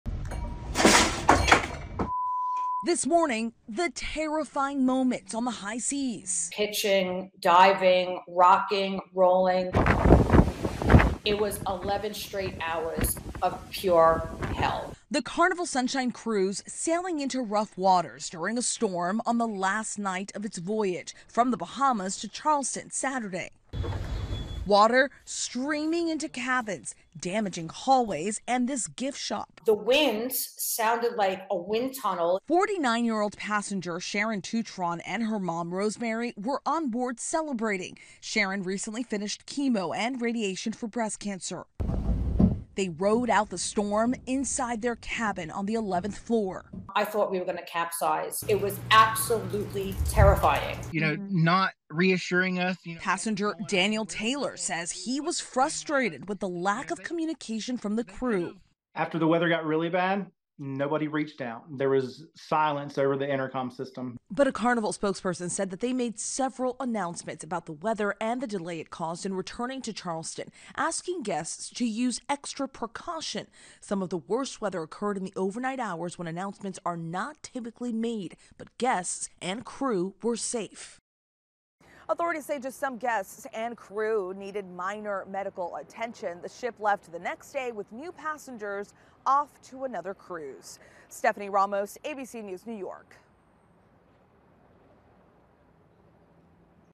Passengers left terrified as cruise ship sails into violent storm
A Carnival cruise ship sailed into a violent storm with giant waves crashing into the ship and wind gusts up to 80 mph. FULL STORY